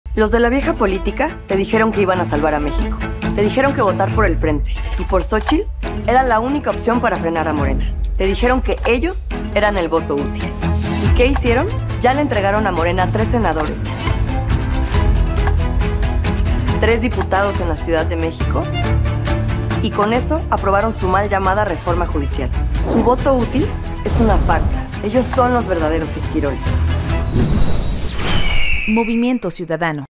El PRIAN: Los esquiroles de Morena.
El "voto útil" que el PRIAN te pidió en campaña fue una farsa. Le dieron sus senadores y diputados a Morena para aprobar la ReformaJudicial. Aquí te lo explicamos.